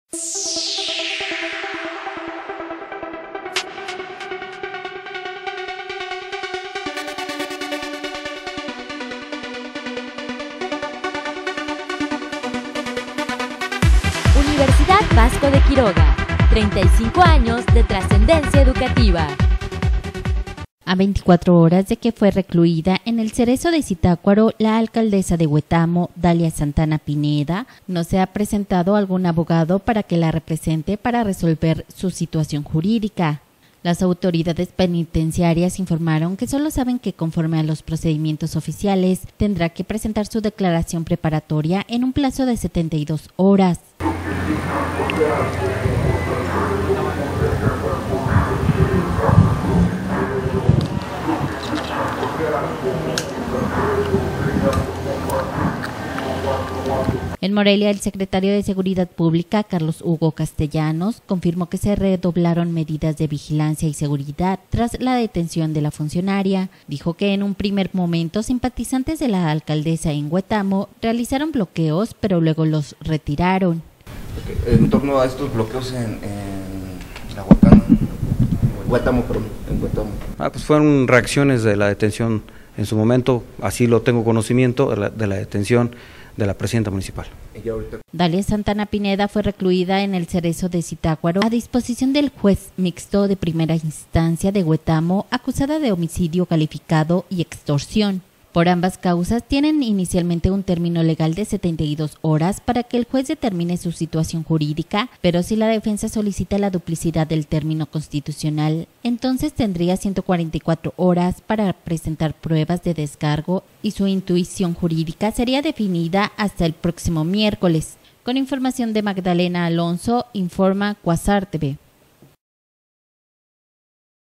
Primer día de alcaldesa de Huetamo en prisión
A 24 horas de que fue recluida en el CERESO de Zitácuaro, la alcaldesa de Huetamo, Dalia Santana Pineda no se ha presentado algún abogado que la represente para resolver su situación jurídica…